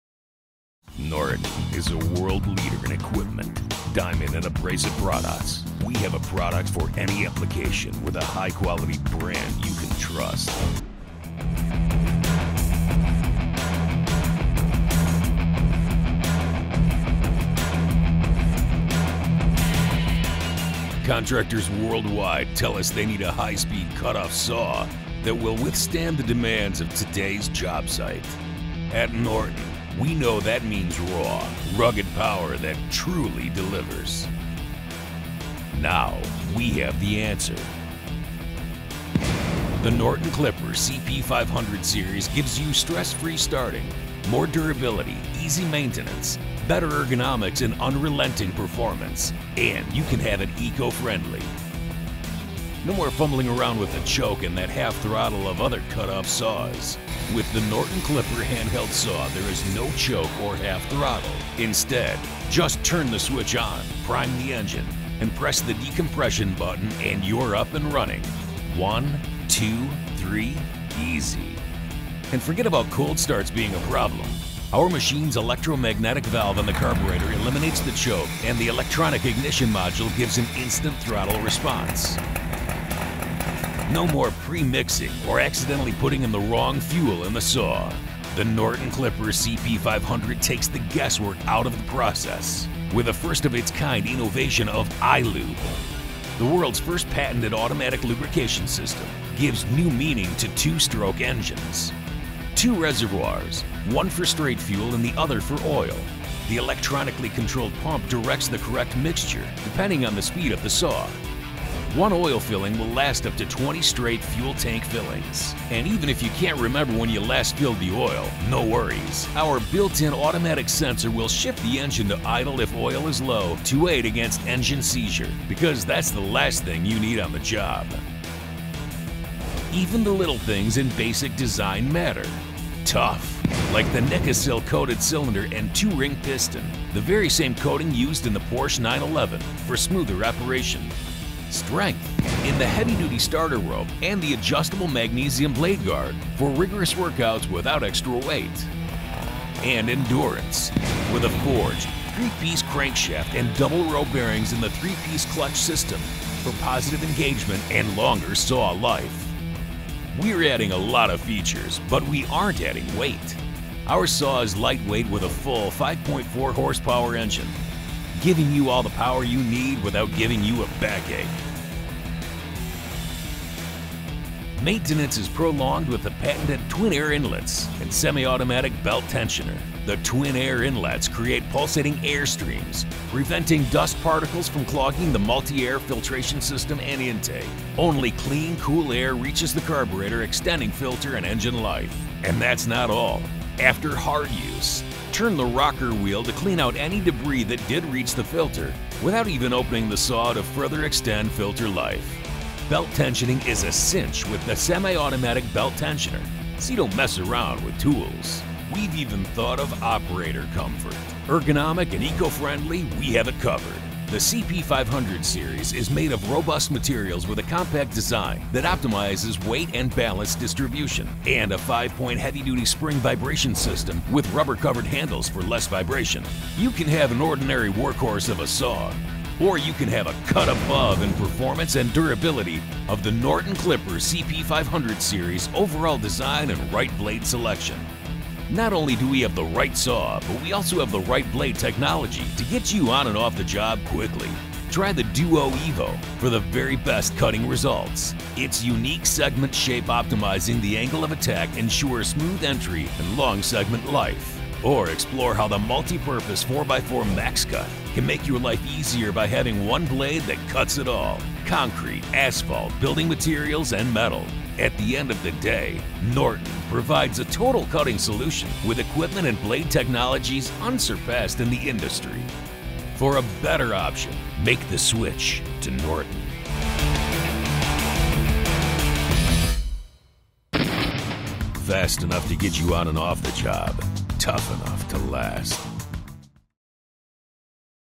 Norton Clipper High-Speed Gas Saws
These saws are built to withstand the demands of today's jobsite with raw, rugged power that truly delivers. Finally a saw that is a cut above ordinary!